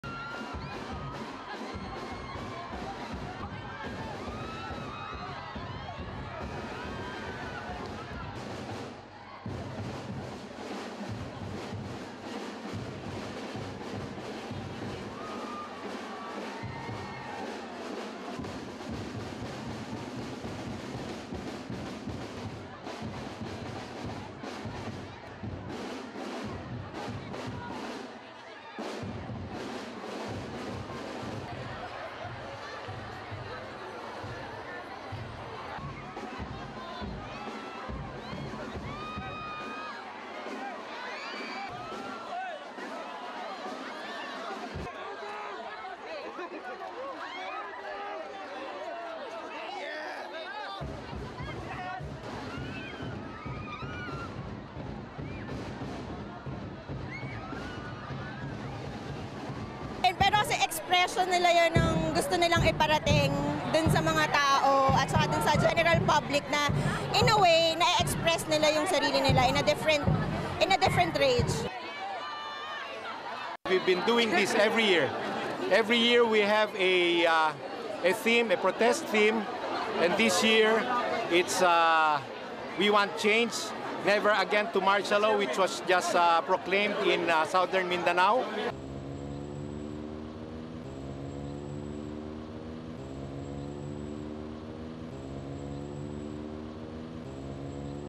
Annual Oblation run by naked students at University of Philippines
1. Wide of university building, students gathered on steps outside cheering
2. Mid of naked runners walking down steps of campus, wearing masks and some holding up placards
3. Wide of naked runners amid crowd
4. Mid of naked runners amid cheering crowd, holding up placards with various messages
5. Various of crowd, some taking pictures or videoing event
6. Mid of runners' placards reading (English): "Protect the Environment" and "Stop the Killing of Journalists!"
7. Wide of campus hallways filled with students watching the event
8. Reverse shot of naked runners standing in front of cheering crowd
9. Mid of crowd cheering
10. Mid of naked runners filing past crowd, tilt up to placard reading (English): "Stop the Killing of Journalists!"
11. Mid of runners going inside building past cheering crowd
12. SOUNDBITE: (English/Tagalog) Cad Ramirez, student:
"It is their form of expression, to communicate with the general public. In a way they are expressing themselves in a different range."
13. Mid of crowd watching
14. SOUNDBITE: (English) Armand Padilla, organiser from Alpha Phi Omega Fraternity:
"We've been doing this every year. Every year we have a theme, a protest theme, and this year it's: "We want change, never again to martial law," which was just proclaimed in southern Mindanao."
15. Wide of main building in university campus with statue in the background
16. Mid of statue, the "Oblation", of which the run is named after
STORYLINE:
For the second time in a week, students at the University of the Philippines took part in the annual "Oblation run" on Tuesday, running naked through campus past crowds of cheering students, media and the general public.
The event is a yearly ritual organised by the Alpha Phi Omega Fraternity, whose members volunteer to do the daring task.
The second run, after last Friday's, was done at the main campus of the university in suburban Diliman.
This year, about 30 members of the fraternity volunteered to run wearing nothing except masks, some carrying placards of different issues they were trumpeting.
The issues ranged from climate change to the recent massacre of journalists and civilians, as well as the martial law imposed in the province where it happened.
A mixture of screaming female students and curious onlookers lined up in the corridor to watch or take pictures of the naked students as they ran past.
The naked run ritual is said to have been inspired by the university's iconic symbol, the Oblation, a bronze sculpture of a nude man, with arms outstretched and face looking up to the sky.